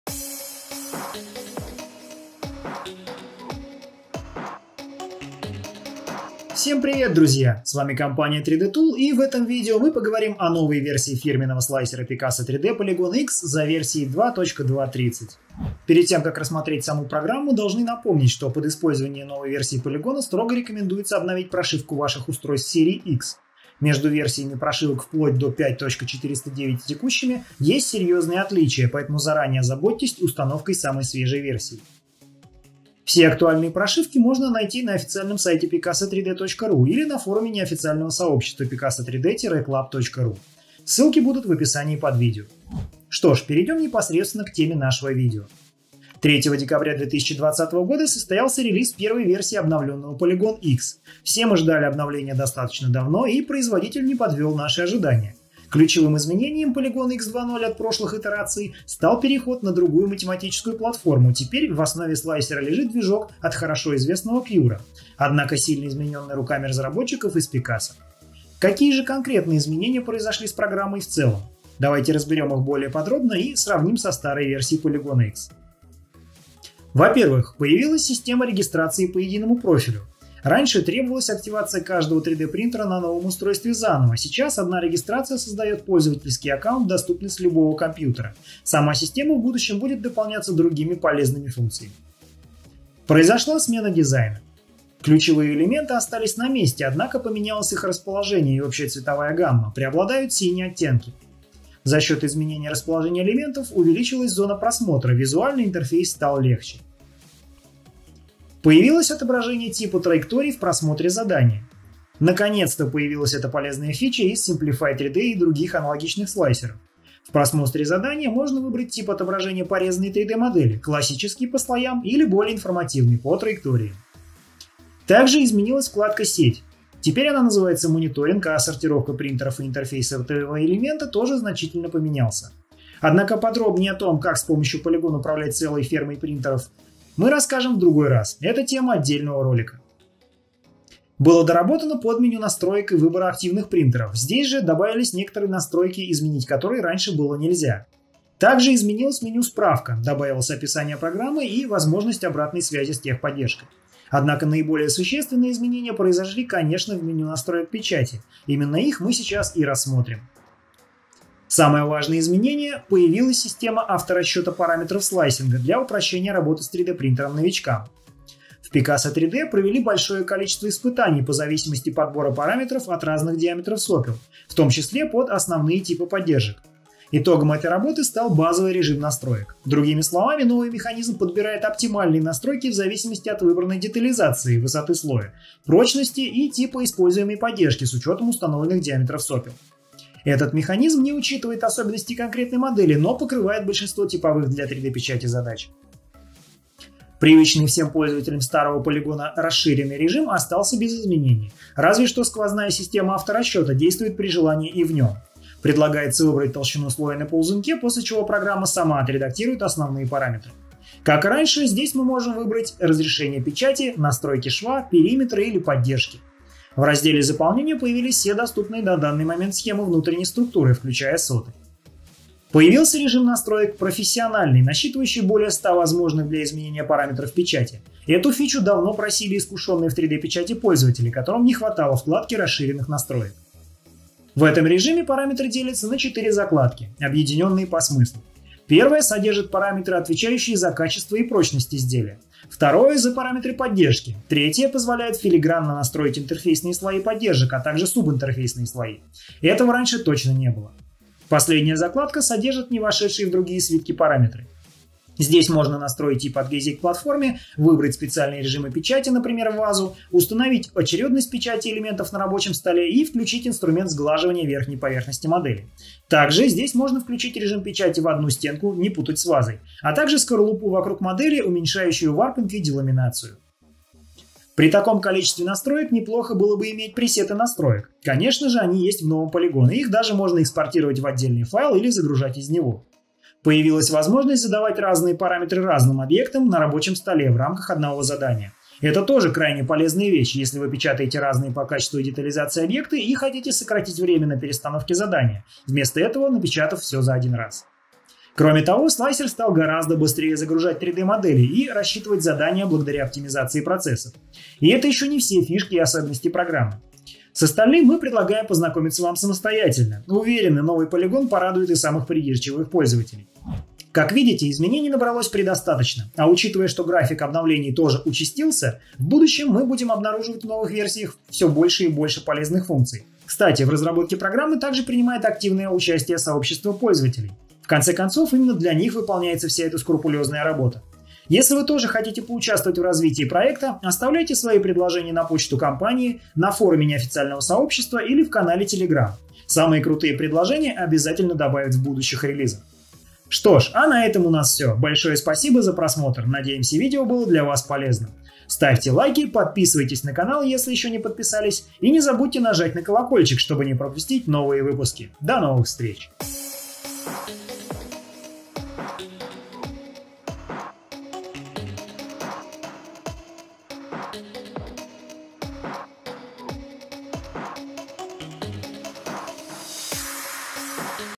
Обзор обновлений слайсера Polygon X от PICASO 3D | Новый релиз 2021 | Официальный дилер 3Dtool
Обновление от 11.02.21

Список того, что вы найдёте внутри:
— Новая система регистрации/активации программы.
— Обновлённый интерфейс.
— Ускоренный процесс генерации заданий на печать.
— 3 режима подготовки заданий на печать: от режима «без цифр» до режима с более, чем 100 параметрами.
— Пользовательские наборы настроек, которые можно скачивать и передавать другим пользователям.
— Механизм автоматического подбора настроек под любые диаметры сопел (представленные PICASO 3D официально).
— 4 автоматически настраиваемых типа поддержки.
— Уникальные настройки для отдельных моделей в рамках одного задания на печать.
— Новые режимы и функции: Ваза, Сглаживание, Реверсивная модель, Защитный щит, Адаптивный слой.
— Исправление ошибок прошлых версий.

Наш офис (демонстрационный зал) находится по адресу: г. Москва, улица Дорогобужская д.14 стр.4, офис 302 , тел.: и